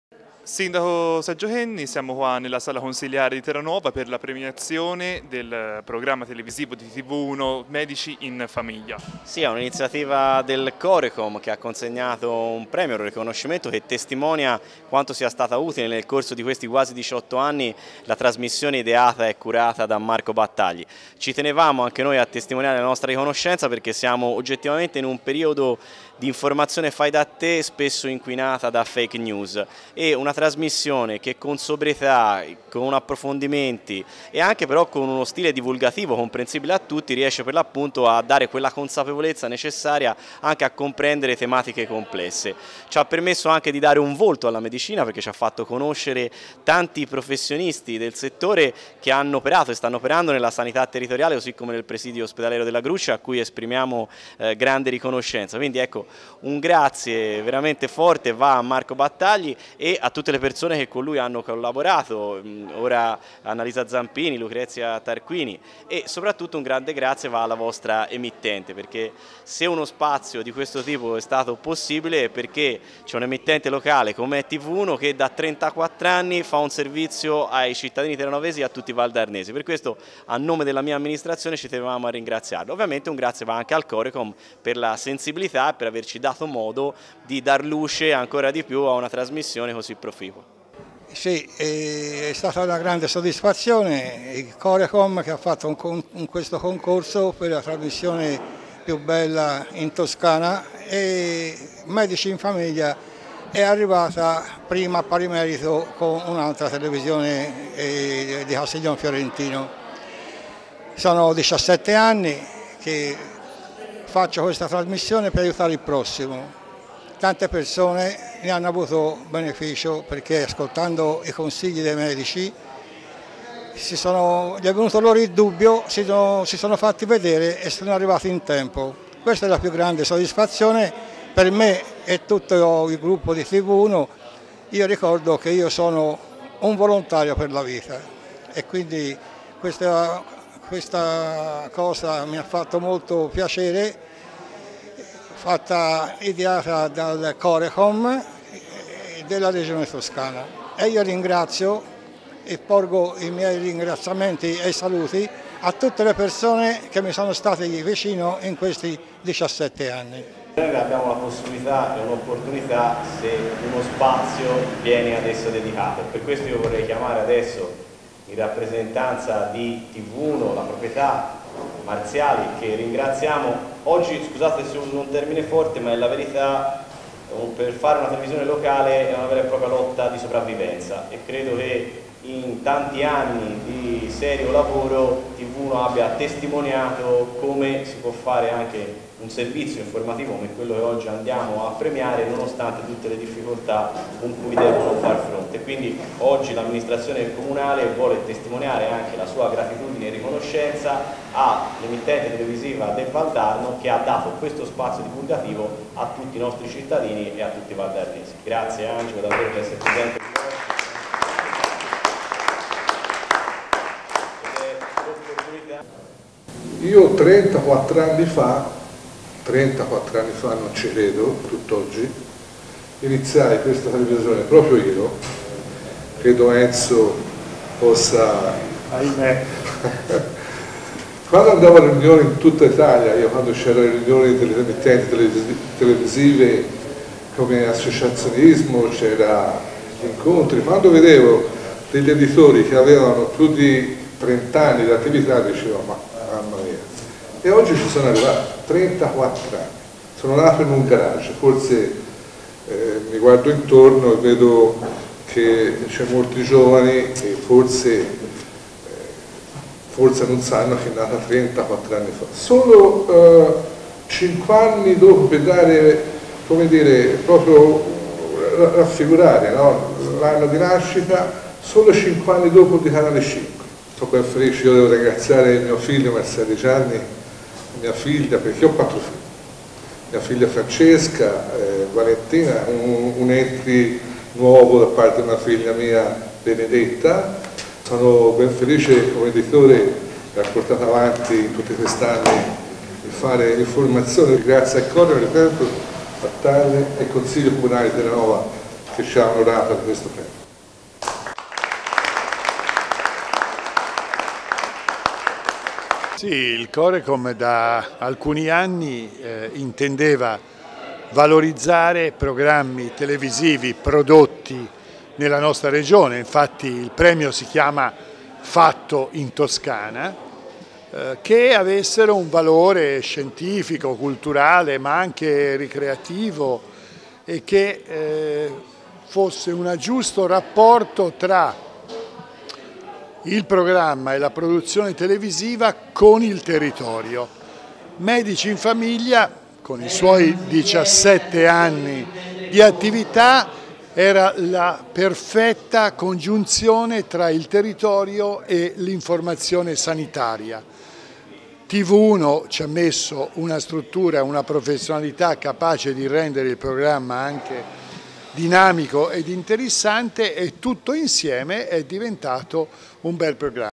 Comune di Terranuova B.ni e Corecom premiano TV1
La nostra emittente televisiva è stata premiata grazie al programma "Medici in famiglia". Il programma sulla sanità locale di TV1 vince il premio di Corecom "Fatto in Toscana".